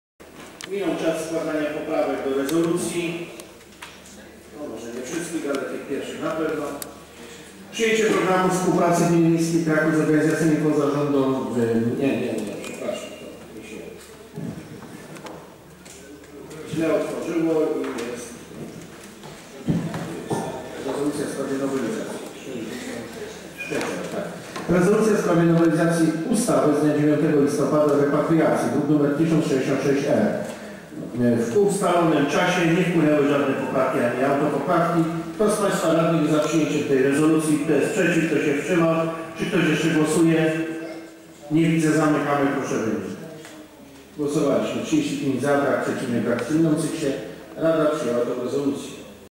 RMK_05-12-2012_39 Rezolucja ws nowelizacji Ustawy z dnia 9 listopada 2000 r. o repatriacji
Sesja LXII Rady Miasta Krakowa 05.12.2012

Rezolucja w sprawie nowelizacji Ustawy z dnia 9 listopada 2000 r. o repatriacji (t.j. Dz. U. z 2004 r. Nr 53 poz. 532 z późn. zm.) w zakresie umożliwienia otrzymania dotacji z Budżetu państwa przez gminę, której rada gminy podjęła uchwałę, w której zobowiązała się do zapewnienia lokalu mieszkalnego określonym imiennie repatriantom i do zawarcia, na czas nieokreślony, umowy nadającej im tytuł prawny do lokalu mieszkalnego

Projekt Grupy Radnych - druk nr 1066-R.